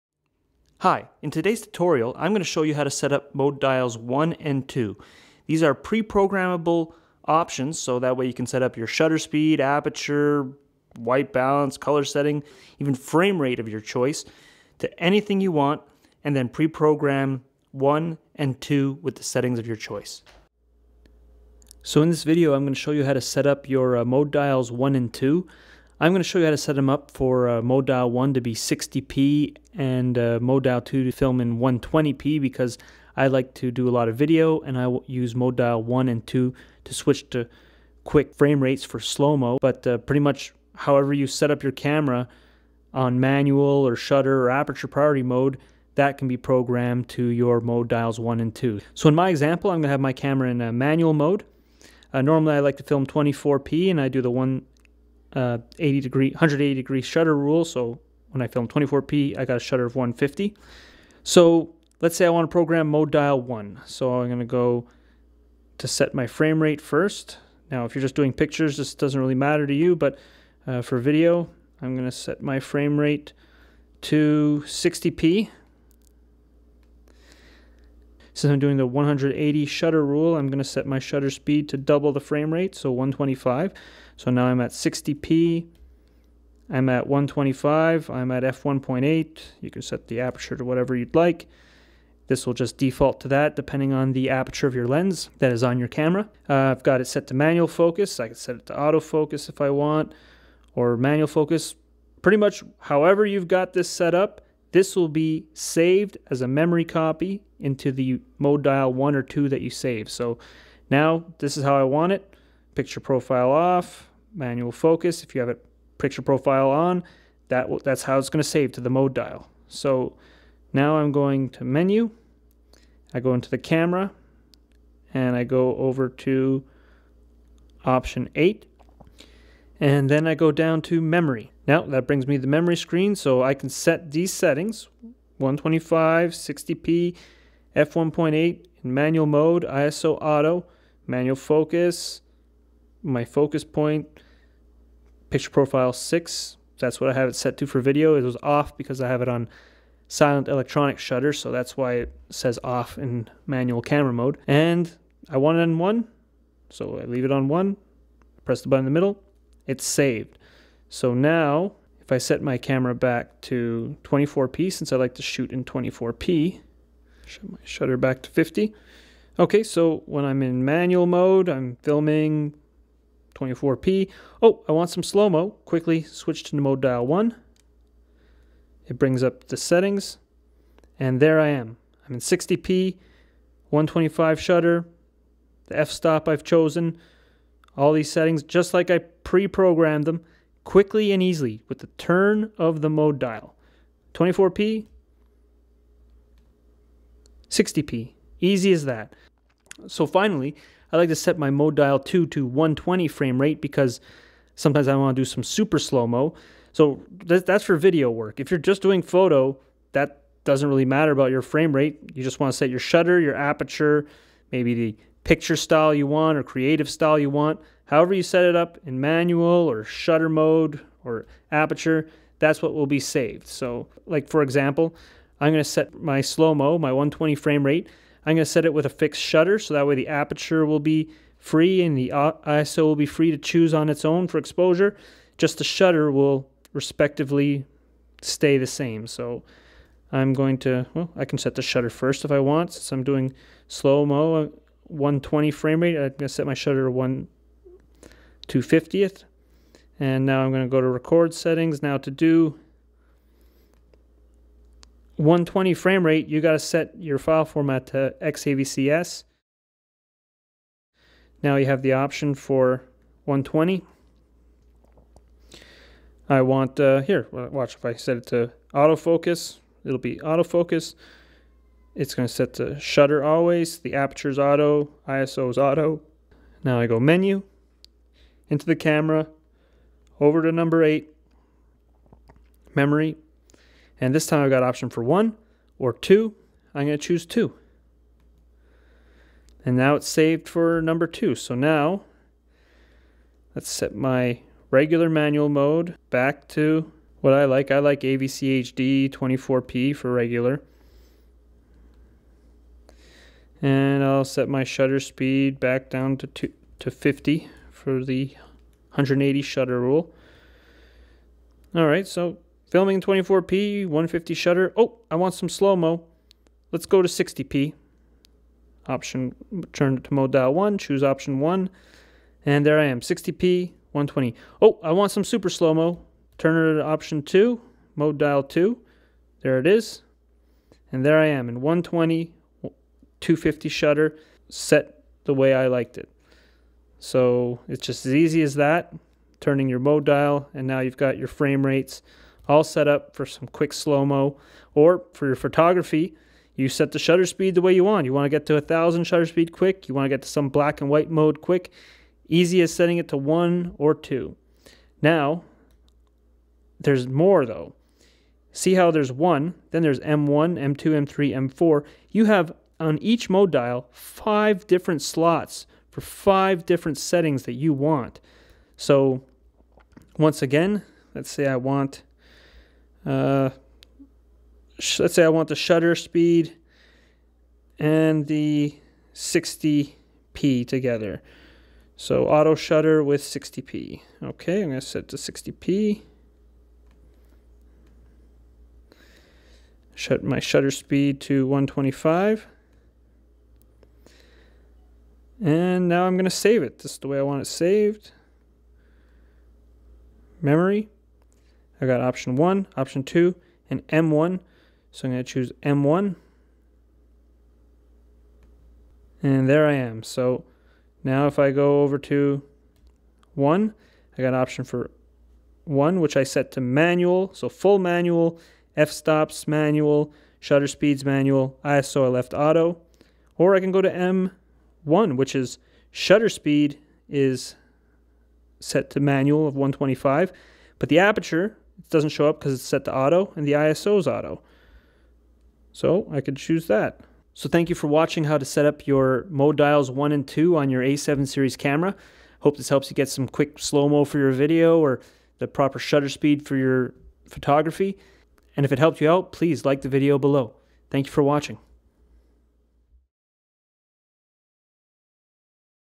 All Sony Alpha A7series and a6300 Tutorial - Mode dial 1 & 2 set up for slow mo video
Tutorial on how to set up mode dial 1 and 2 on all a7 series and
a7series Mark II and a6300.  This version has focus on setting up for slow mo video. I set mode dial 1 to 60p frame rate and 1/125th shutter. And for some camera's that have the option for 120p like A7s, A7sii and a6300 then I set mode dial 2 to 120p frame rate and 1/250th shutter.  I do this so I can quickly access these frame rates for when I want to shoot some video to later convert into slow motion in my video editing software.

You can save a copy of the way you have set up your camera to either mode dial 1 or 2 for quick access.  So what ever you set your Shutter Speed, Aperture, ISO, Creative Style like B&W or Vivid, Picture Profile, Frame Rate like 24p or 60p or 120p, Auto or Manual focus etc... you can save a set up that you like for quick access using mode dial 1 or 2.

I love this feature but it is not very clear how to set up so that is why I made this tutorial.

Here is a link to explain 180 degree shutter rule I mention in the video. It's about what shutter speed to choose depending of your frame rate.